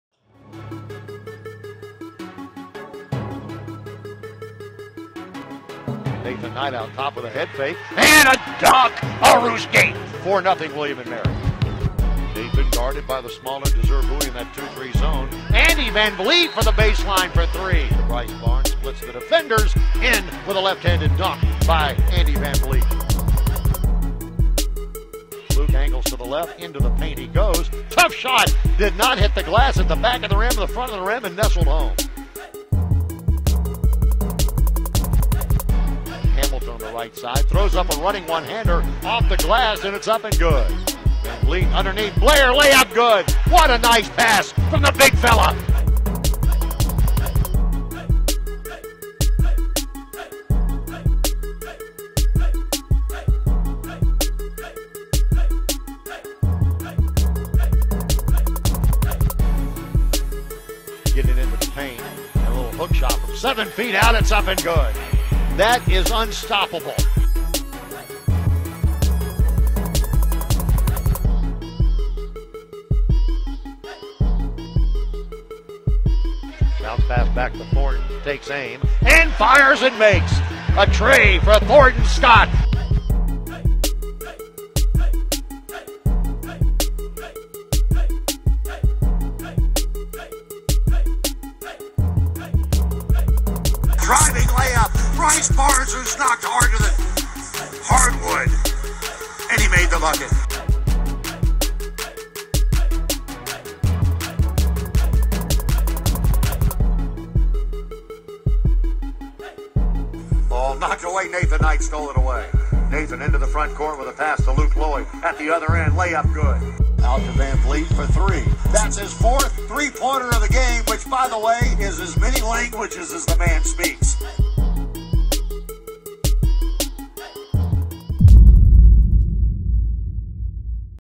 2019-20 William & Mary Men's Basketball Highlights at Hofstra
Check out the highlights from William & Mary Men's Basketball's 88-61 win at Hofstra on Jan. 2.